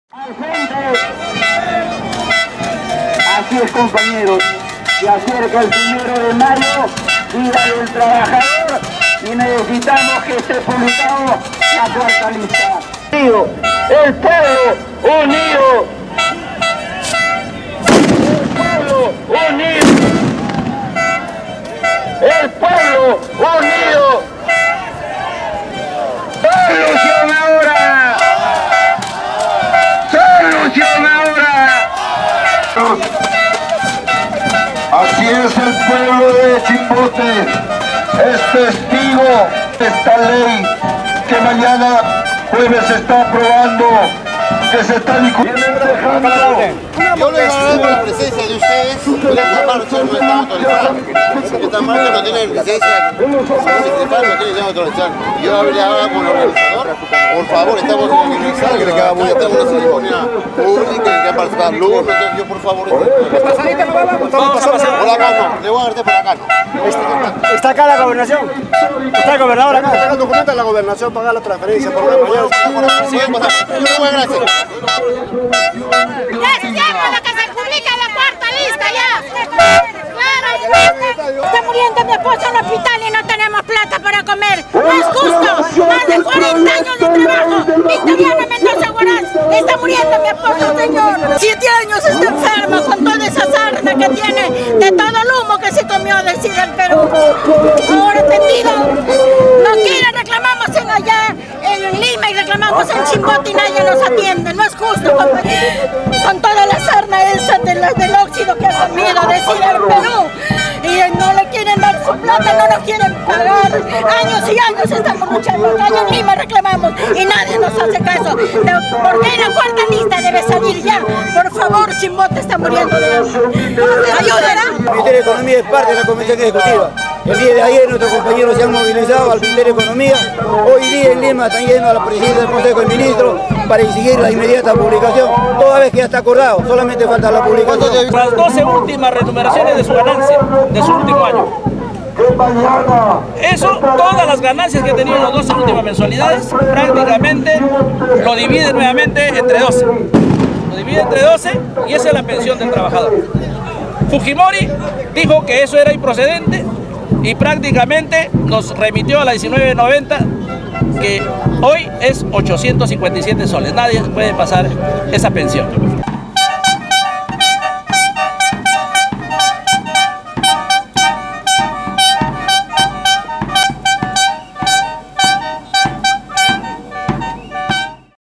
CANAL31 - APROBACIÓN DE UNA PENSION PARA JUBILADOS OPTIMA
Trabajadores despedidos de SIDERPERU exigen aprobación de una pensión justa para jubilados.
Un gran número de manifestantes salió a las calles de Chimbote dando muestras de sus reclamos.